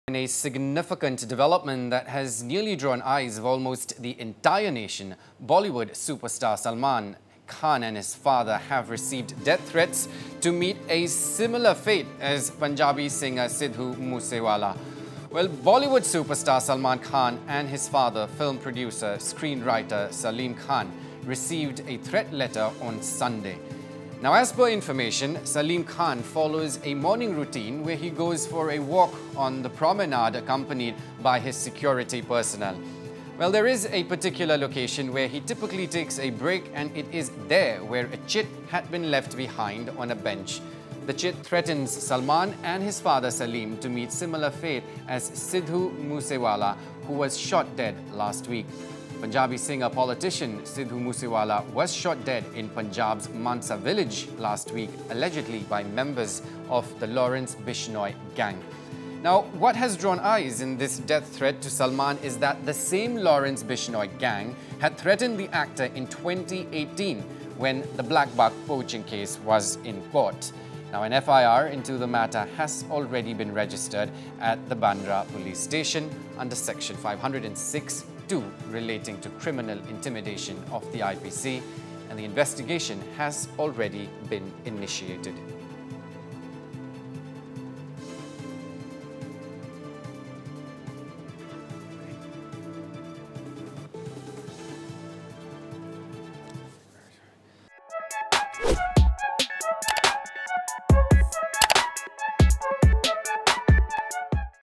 Death threat to Salman Khan and his father
𝐃𝐞𝐚𝐭𝐡 𝐭𝐡𝐫𝐞𝐚𝐭 𝐭𝐨 𝐒𝐚𝐥𝐦𝐚𝐧 𝐊𝐡𝐚𝐧 𝐚𝐧𝐝 𝐡𝐢𝐬 𝐟𝐚𝐭𝐡𝐞𝐫 | Bollywood superstar Salman Khan and his father have received death threats on Sunday to meet a similar fate as Punjabi singer Sidhu Moose Wala who was shot dead last week.